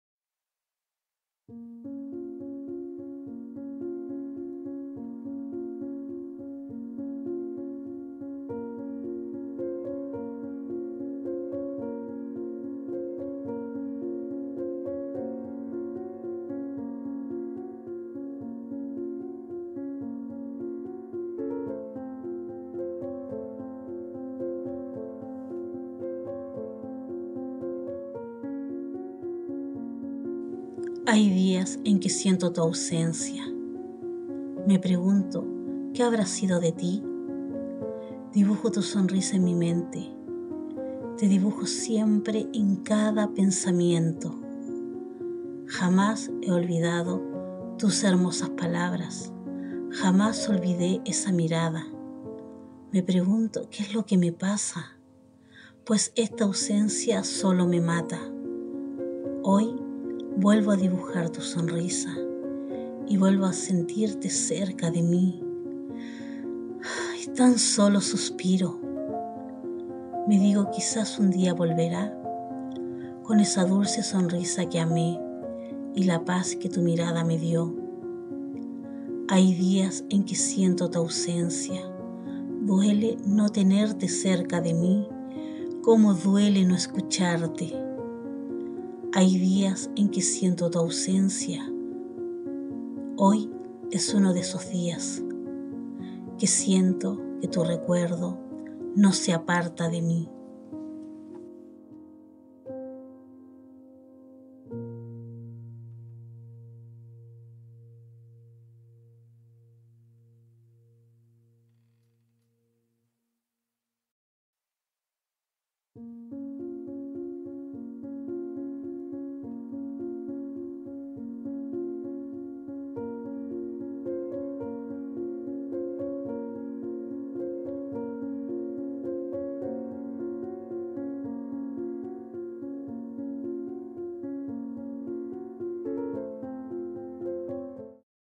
Hay días en que siento tu ausencia - Prosa
Prosa que he escrito hace un par de años.
Me gusta escribir sin ser escritora. Hay veces que escribo de lo que me han contado, vivencias ajenas, muchas veces dolorosas. Otras veces escribo acerca del optimismo y la superación. No todo tiene que ser triste.
La vida en si tiene matices. Prefiero los colores alegres a esos colores opacos.